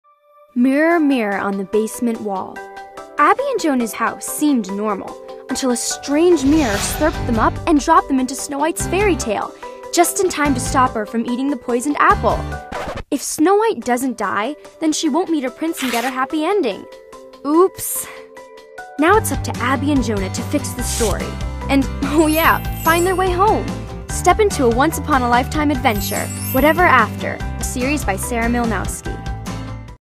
Whatever After (Books 1-10) by Sarah Mlynowski
Follow Abby and Jonah through the enchanted mirror in their basement into fairy tale worlds. Whatever After--the hilarious and magical series by Sarah Mlynowski!